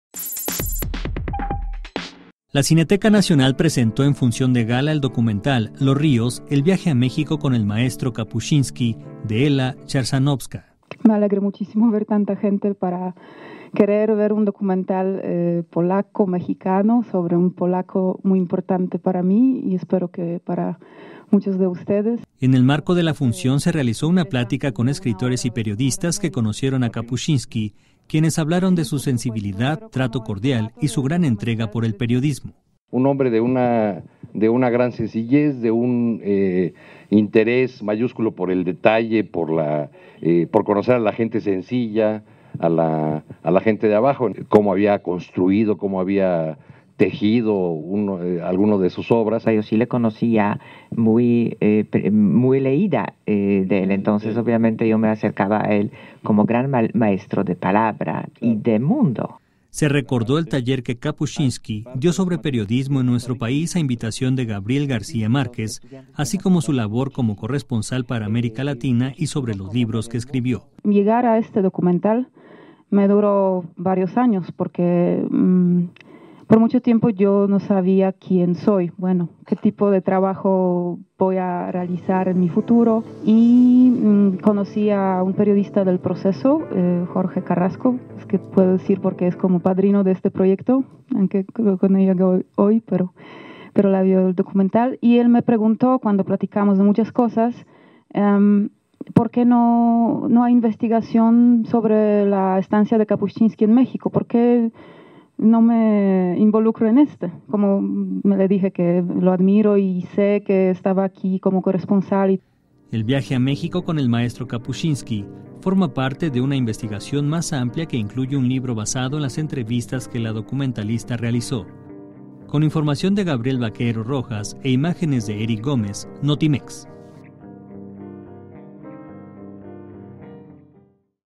Kapuscinski llega a la cineteca a través de un documental
"Los ríos. "El viaje a México con el maestro Kapuscinski", de la polaca Ela Chrzanowska, rememora al teórico  el periodismo a través de interesantes pasajes cinematográficos